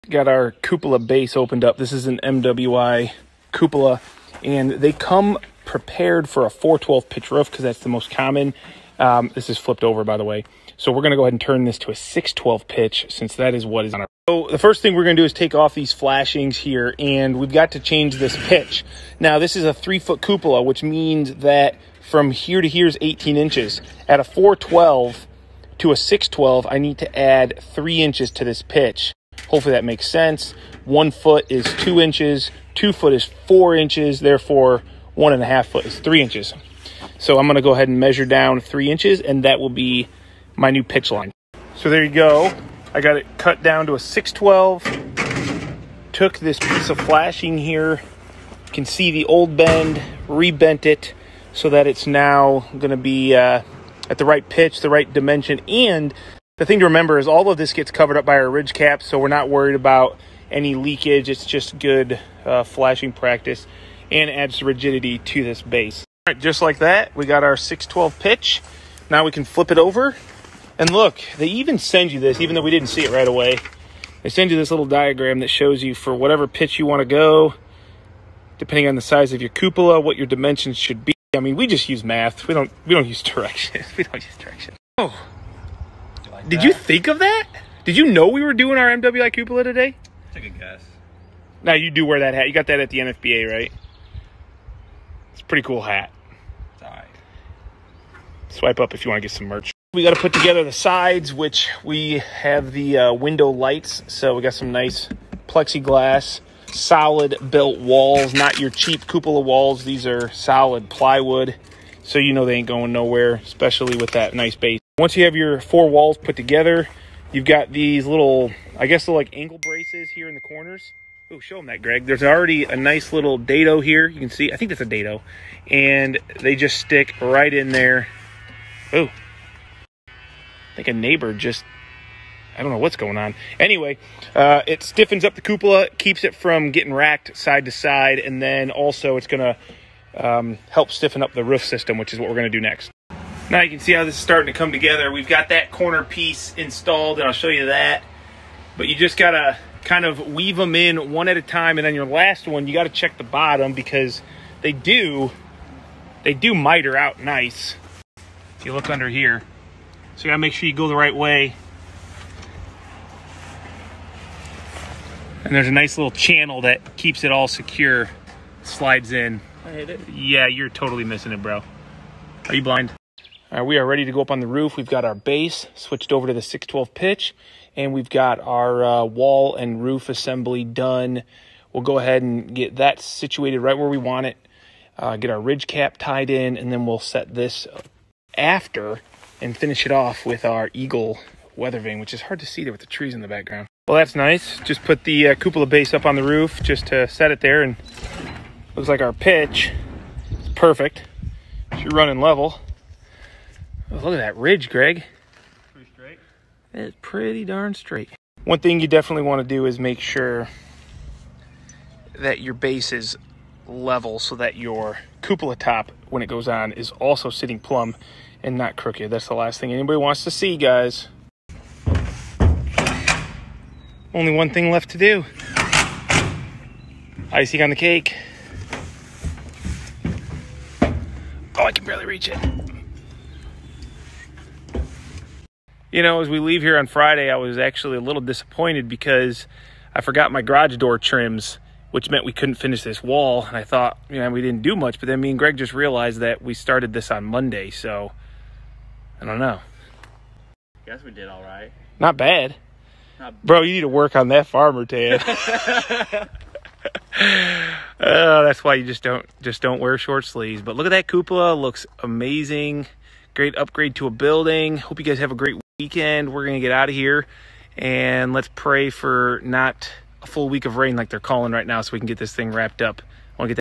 Cupola Base/Cupola Sides Assembly (Overall Assembly)
Learn from RR Builders about MWI Cupola base assembly & adjusting pitch as well as overall cupola installation!